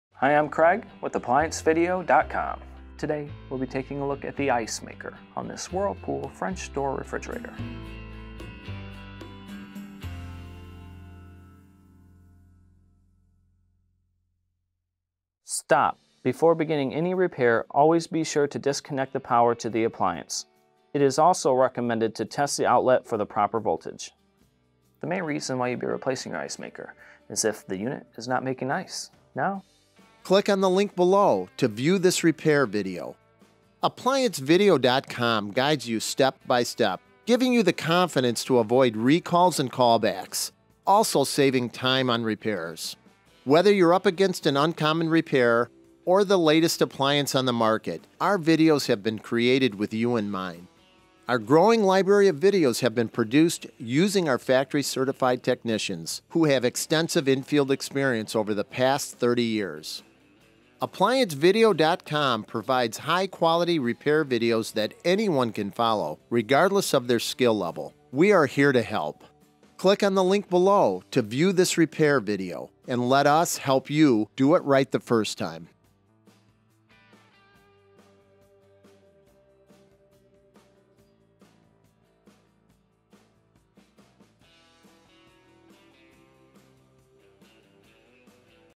Whirlpool Refrigerator - No Ice Production - Ice Maker Assembly Repair
This is a preview of a video that shows you how to Repair a *WRF540CWHV01 Whirlpool Refrigerator W10884390 Ice Maker Assembly*
*Symptoms may include:* No Ice Production

This *Whirlpool* repair could apply to multiple brands: such as Whirlpool, Amana, Maytag, Roper, KitchenAid, Jenn-Air, Kenmore, Magic Chef, Admiral, Norge, and Crosley